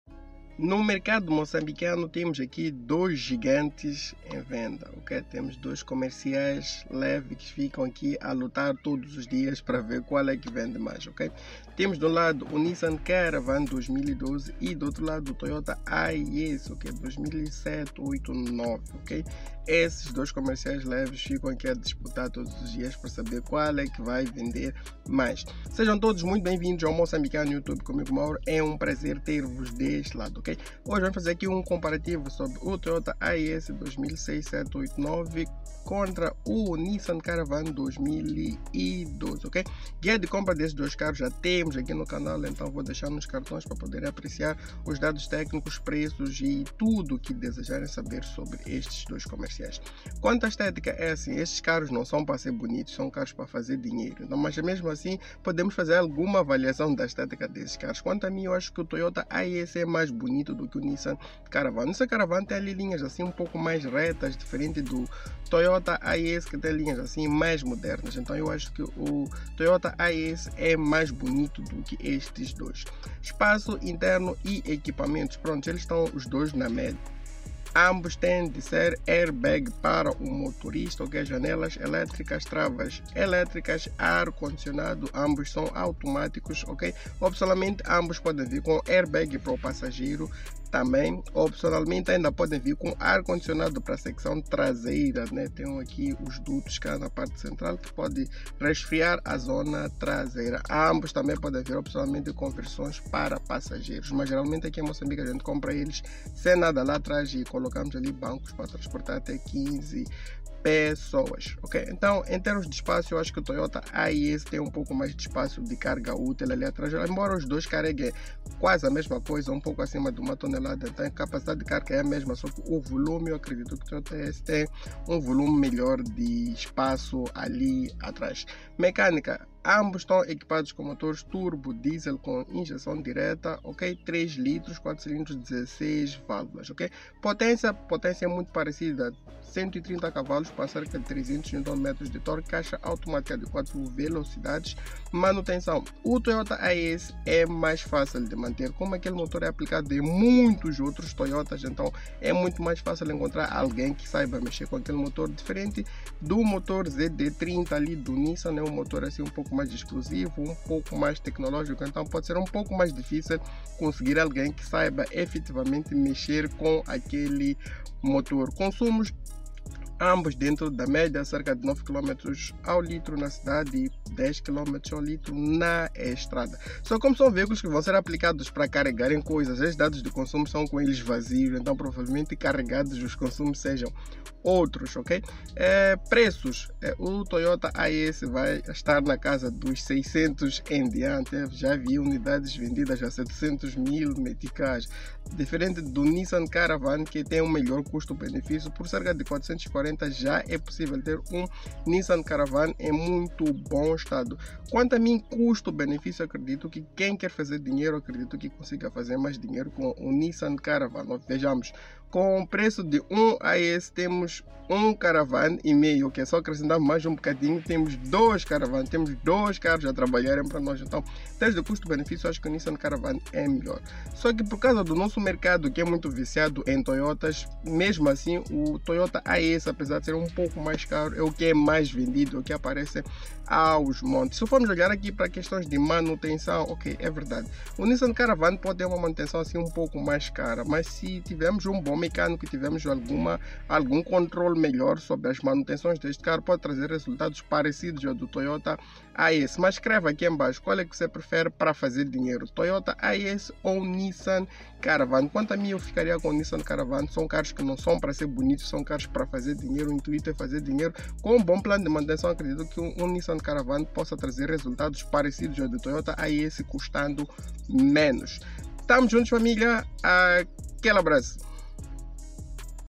Comparativo Toyota Hiace 3.0 Diesel VS Nissan Caravan 3.0 Diesel
E ai familia, tudo bem convosco? Comigo tudo bem.

Neste episódio trago vos comparativos entre as minivans Toyota Hiace e Nissan Caravan ambos nas versões 3.0 turbo diesel.

São dois comerciais leves muito bem aceites em Moçambique. Sendo sempre as mais vendidas por aqui.

Trazendo bom espaço interno, boa lista de equipamentos, manutenção e consumos aceitável e alto nível de lucratividade.